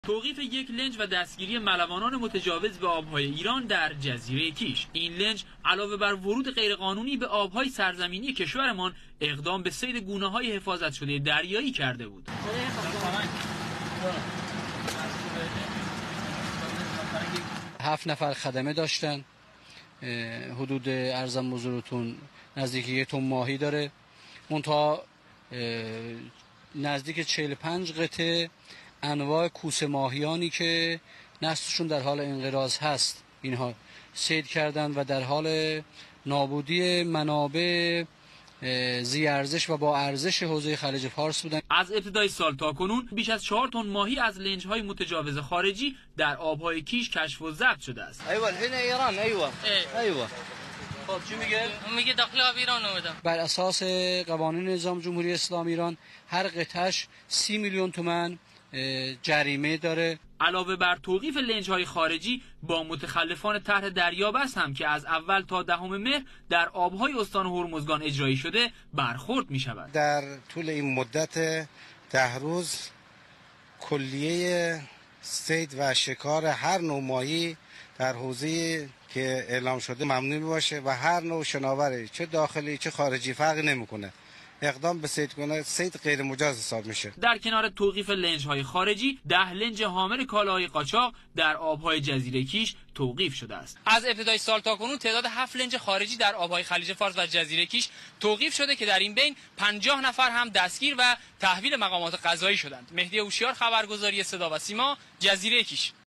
Iran Border Guard police arrested Arabic countries fishermen, Persian Gulf بازداشت ماهيگيران بيگانه
September 24, 2017 (Persian calendar 1396/7/2)

Iran Border Guard police arrested Arabic countries fishermen, Persian Gulf
پليس مرزباني بازداشت ماهيگيران بيگانه خليج فارس ايران